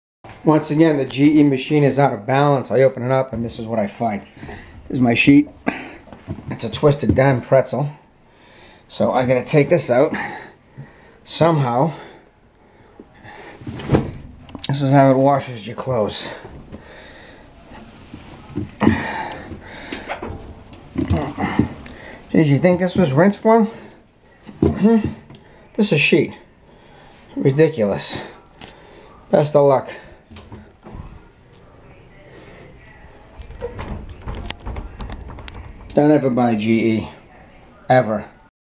GE WASHER FAILS AGAIN!! 2018 WASHER
GE Washer constantly out of balance.
Still under warranty, GE Fails to address the issue..